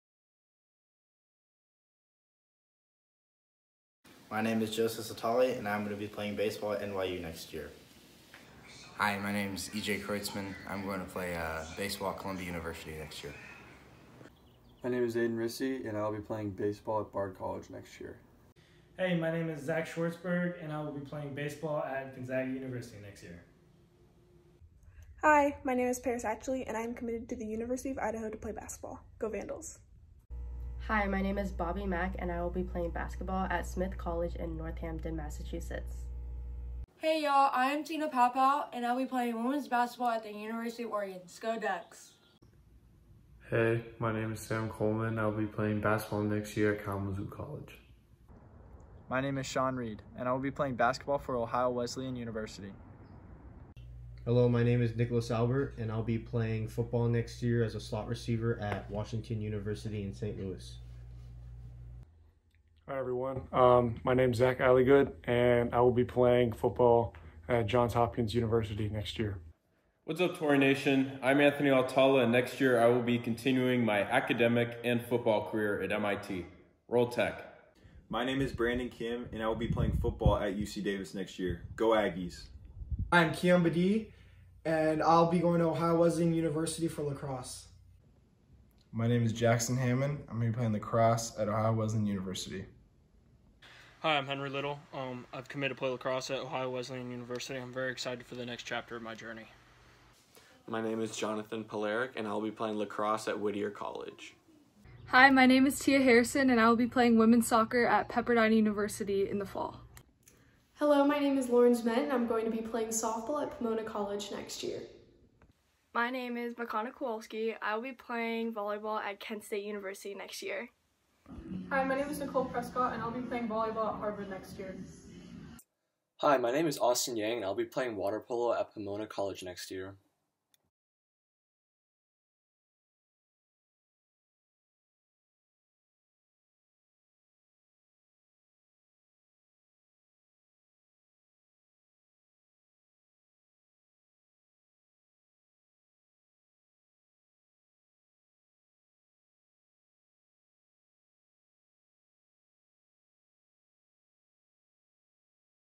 La Jolla Country Day Class of 2020 Student-Athlete College Commitments
LJCDS is very proud of the many student-athletes who will competing in college next year. Check out this video to see their commitment announcements.